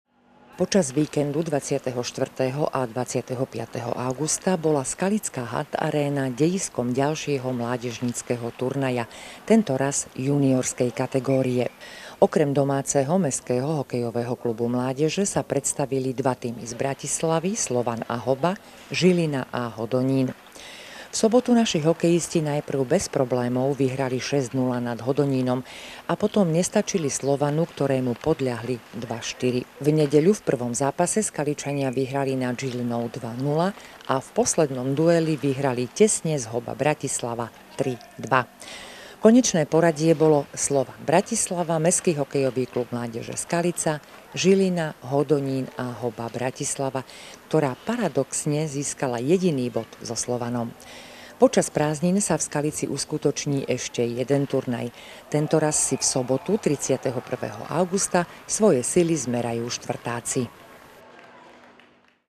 Turnaj juniorov
V Hant aréne sa konal turnaj hokejistov juniorov